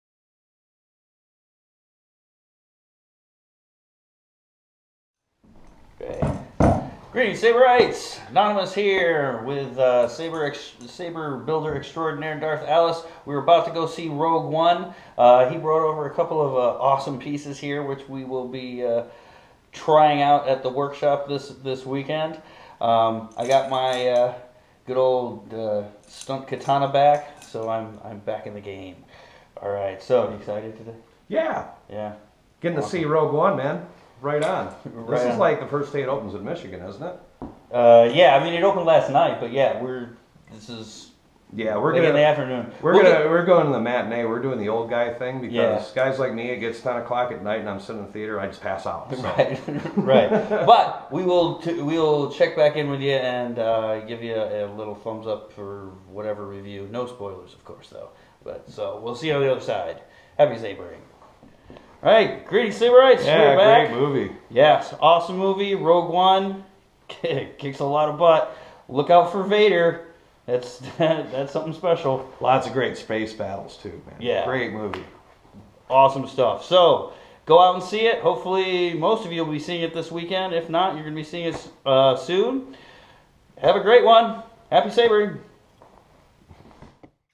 Going to Rogue One with Darth Alice
Noymous and Sabersmith extrodinare Darth Alice go to see "Rogue One".

Protect yourself!